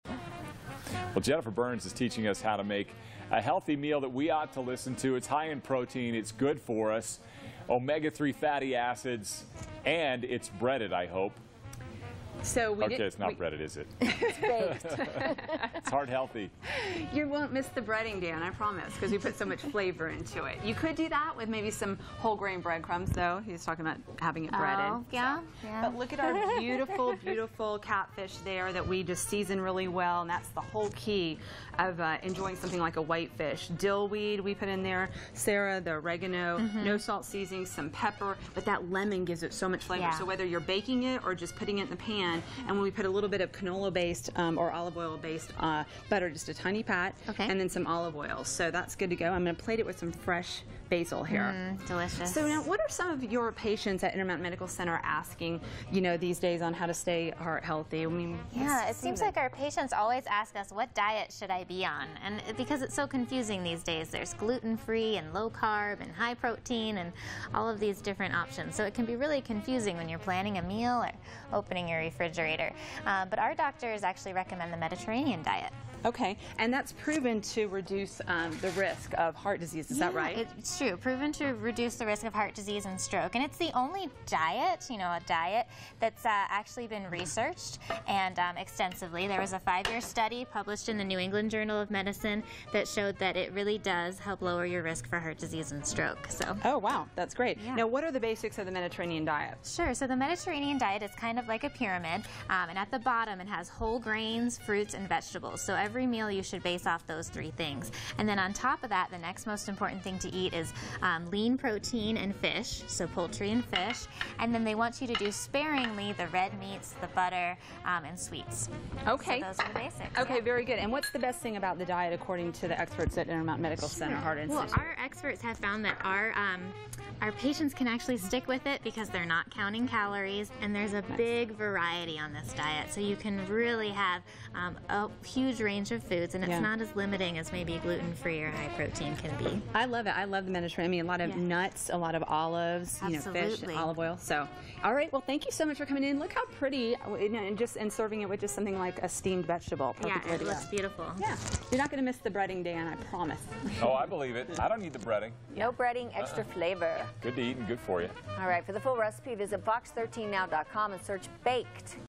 Baked Catfish
Join  Fox 13 News Jennifer Burns and Sarah Young from the Intermountain Medical Center Heart Institute as they teach you how to cook a healthy and delicious catfish.